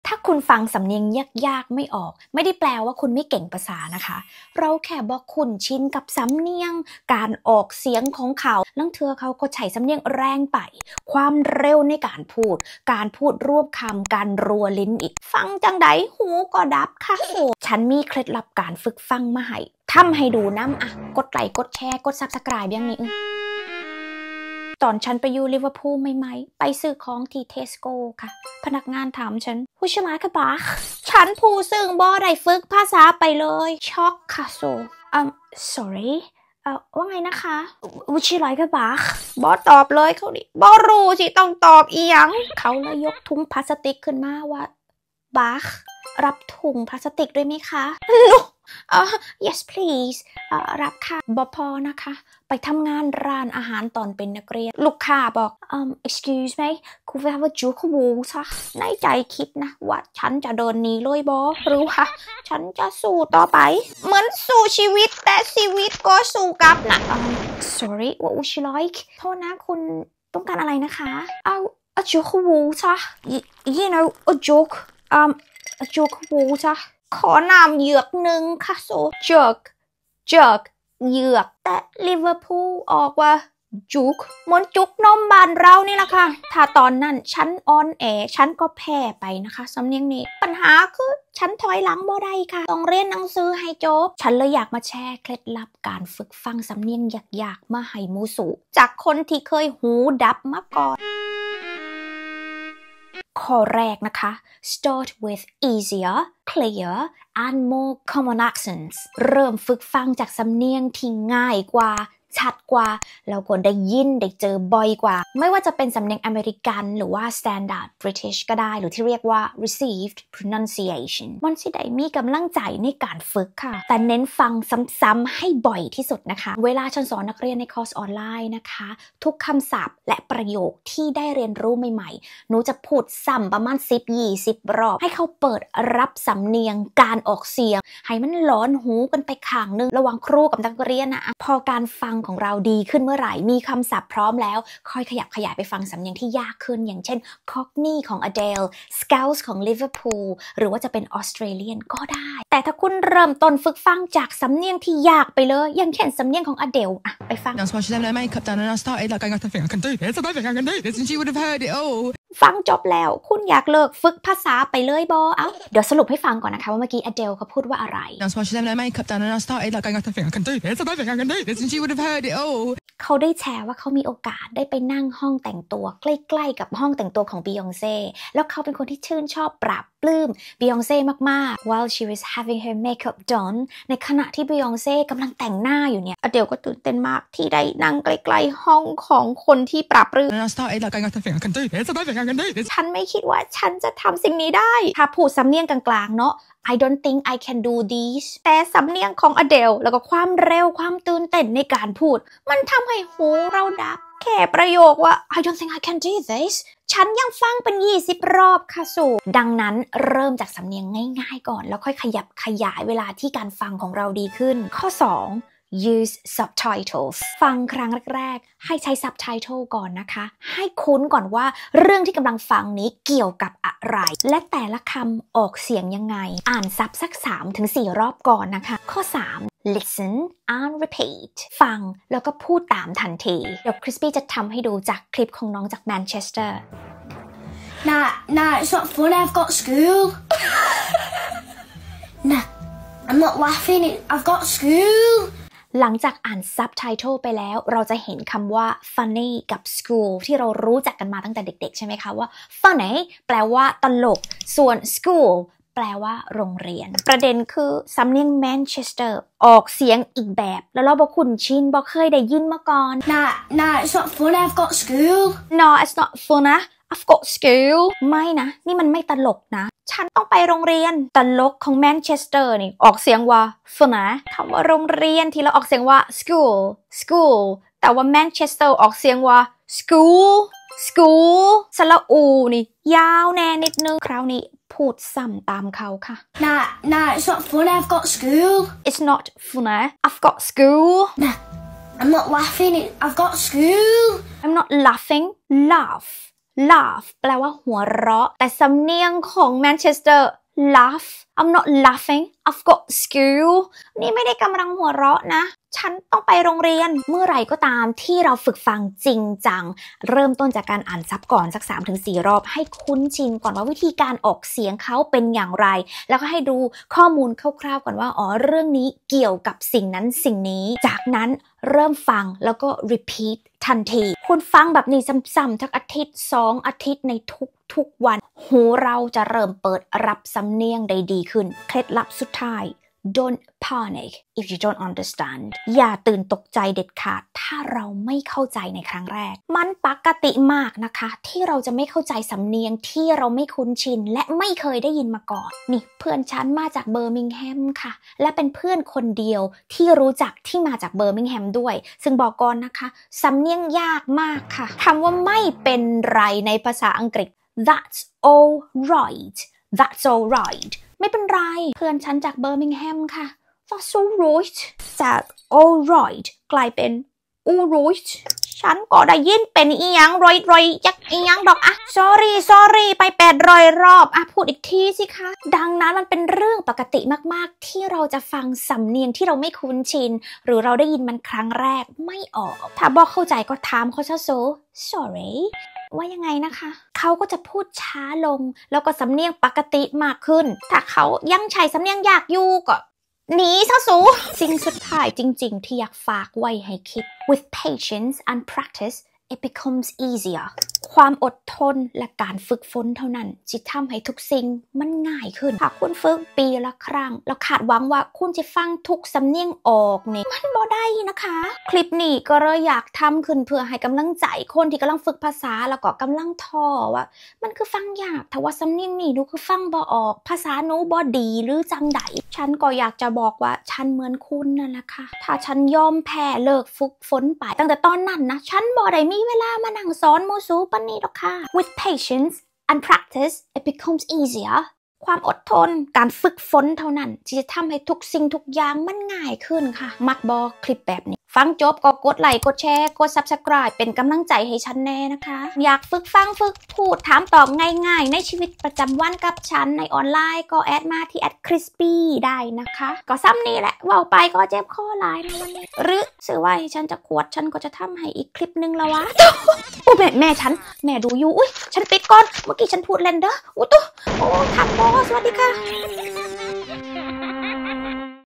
การฟังสำเนียงยากๆ ไม่ออก ไม่ได้หมายความว่าคุณไม่เก่งภาษานะคะ 🇬🇧. (เคล็ดลับการฟังภาษาอังกฤษ)
การฟังสำเนียงยาก ๆ ไม่ออก
ไม่ได้หมายความว่าคุณไม่เก่งภาษานะคะ  🇬🇧

•เราเเค่ไม่คุ้นกับสำเนียงเขา
•สำเนียงเเรงไป
•ความเร็วในการพูด
•การรวบคำ รัวลิ้น

ทอนั้นเองจ้า

ตอนฉันไปเรียน Liverpool ใหม่ๆ
ไปซื้อของ Tesco
พอจะจ่ายเงินเขาถาม.

•Would you like a bag?
รับถุงพลาสติกด้วยไหมคะ

เเต่ด้วยสำเนียงคำว่า Bag ของ Liverpool
“คือ... เเบ็คคคคคคคคค”

เสียง “คึ” เขา ลากยาวมาก
ฉันก็บอเข้าใจ..
ได้ Sorry? อยู่ 80 รอบให้เขาพูดใหม่😂

หรือตอนทำงาน part time ร้านอาหารเเรกๆ

ลูกค้าขอน้ำเหยือกหนึ่ง
•Can we have a jug of water?

เขาออกเสียงว่า
“อะจุค ออก โวทะ”

ฉันถามว่า ...
ฉันเดินหนีได้บอ ก่อนน่ะ😂
มันยากอีหลีเดะจ้า

ฉันเลยอยากให้เคล็ดลับการฝึกฟังสำเนียงยากๆ
จากคนที่เคยหูดับมาก่อนอย่างฉัน

1️⃣ เริ่มฝึกฟังจากสำเนียงที่ง่ายกว่า
      ชัดกว่า เเละ ที่เจอ ที่ได้ยินบ่อยกว่า

เช่น
•American
•Standard British (Received Pronunciation)

เราจะได้ยินบ่อยเลย ค่ะ ตามสื่อต่างๆ
เเต่เน้นการฟัง ซ้ำๆ บ่อยๆ ที่สุด

2️⃣ Use subtitles
ฟังครั้งเเรกๆ ดูซับ ก่อนนะคะ
ฟังเเบบ อ่านซับ สัก 3-4 รอบก่อน

ให้คุ้นก่อนว่า
1.สิ่งที่ฟังเกี่ยวกับอะไร
2.เเต่ละคำ เขาออกเสียงยังไง

3️⃣ Listen and Repeat
ฟังเเละพูดตาม

ถ้าลงมือฝึกฟังจริงจังเมื่อไร
หูเราจะเปิดรับสำเนียงง่ายขึ้นทันที

4️⃣ Don’t Panic if You Don’t Understand
อย่าตื่นตกใจ ถ้าคุณไม่เข้าใจ

มันปกติมากเดะจ้า .
ที่จะไม่เข้าใจสำเนียงที่เราไม่เคยได้ยินมาก่อน

5️⃣with patience and practice, it becomes easier.
ความอดทน เเละการฝึกฝนเท่านั้น
จะทำให้การฟังง่ายขึ้น

มักบอ คลิปเเบบนี้เเมะจ้า
รักเด้อ

สนใจคอร์สภาษาอังกฤษสำหรับคนไม่มีพื้นฐาน
เด้อจ้า